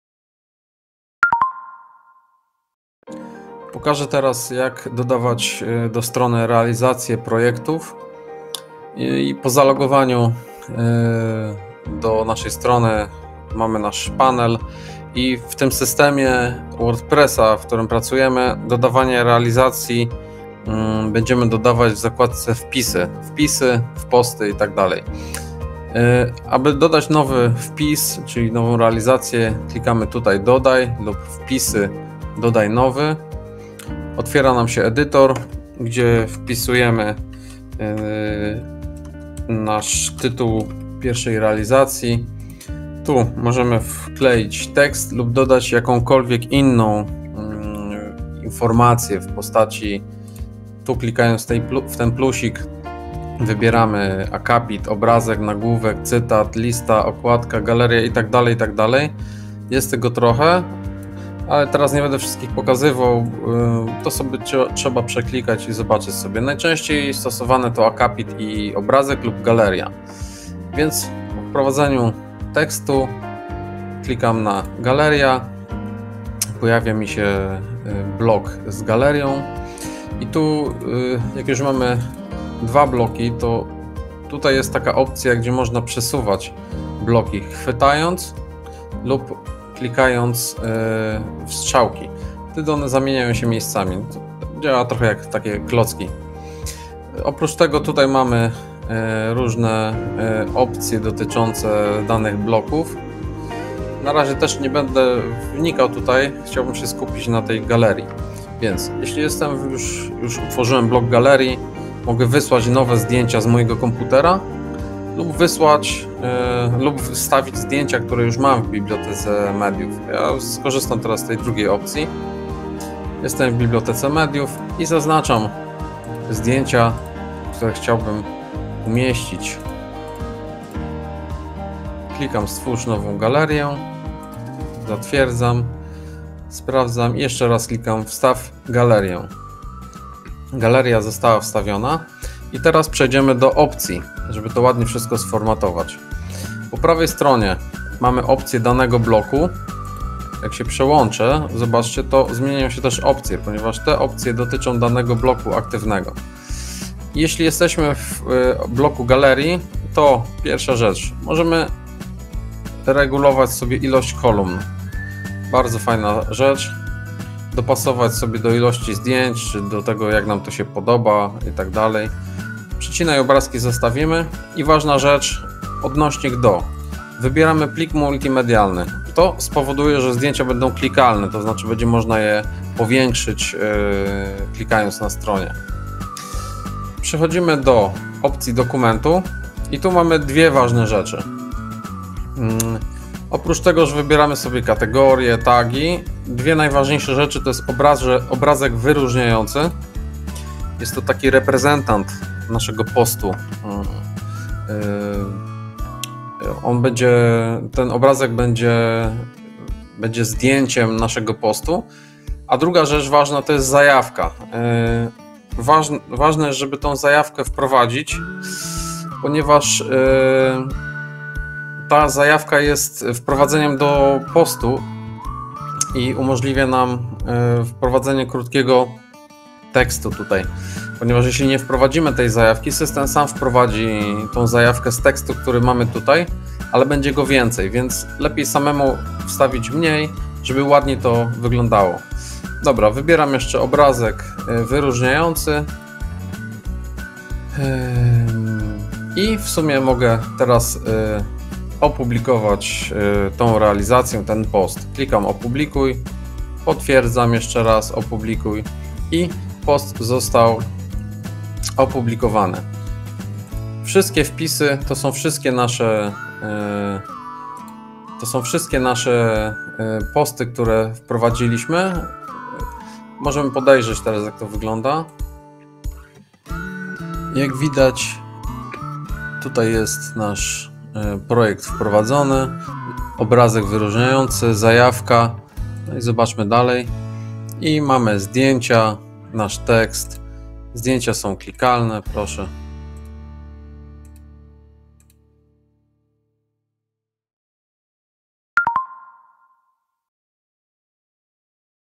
Wstawianie posta/realizacji projektu
Proste wideo o wstawianiu treści  posta.